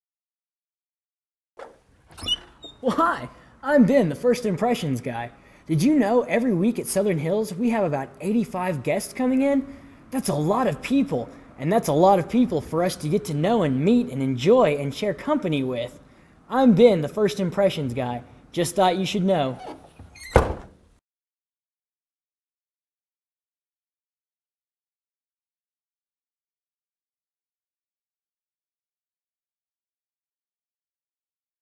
First Impression Guy - 85 Guests
This is a series of videos made to encourage the members of Southern Hills to be a part of the First Impressions team.